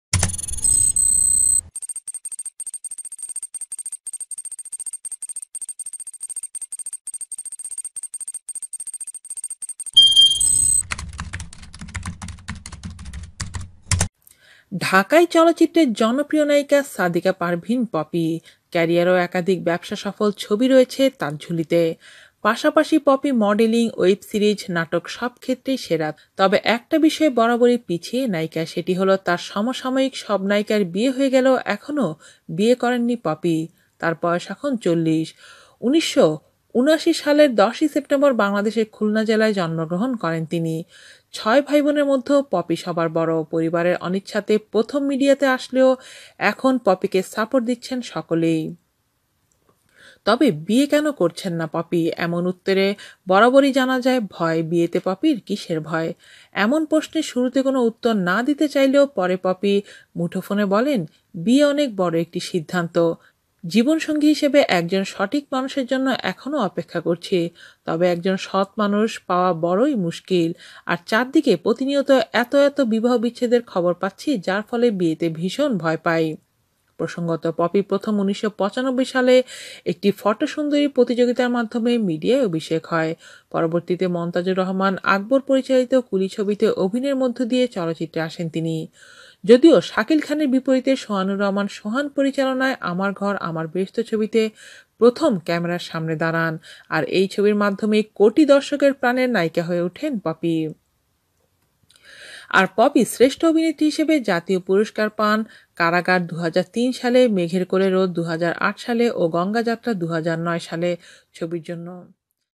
৪০ বছর পার হলেও যার কারণে বিয়ে করেননি কুমারী নায়িকা পপি, নাম জানলে অবাক হবেন আপনিও |BD Actress Popy
৪০ বছর পার হলেও যার কারণে বিয়ে করেননি কুমারী নায়িকা পপি, নাম জানলে অবাক হবেন আপনিও | BD Actress Popy